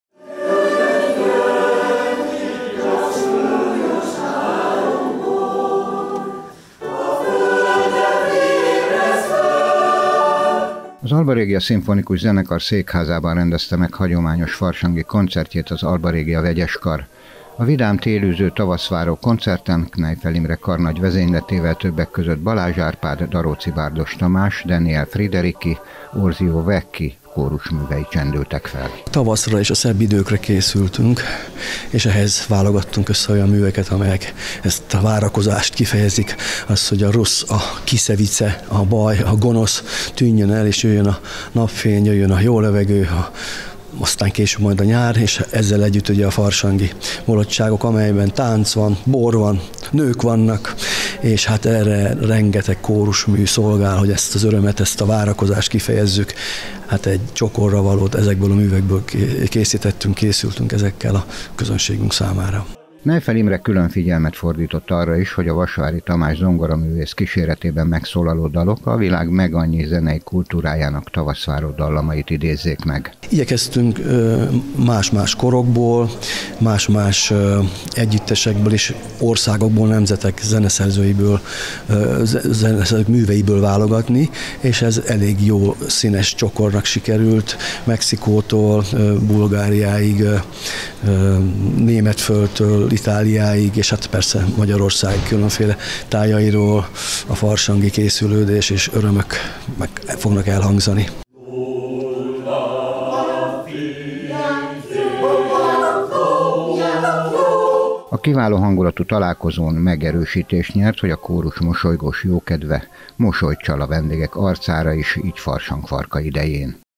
FERGETEGES FARSANG
Fergeteges farsang címmel rendezte meg koncertjét az Alba Regia Szimfonikus Zenekar. Az Arso székházban megrendezett tavaszváró koncerten vidám dalok hangzottak el a világ minden tájáról.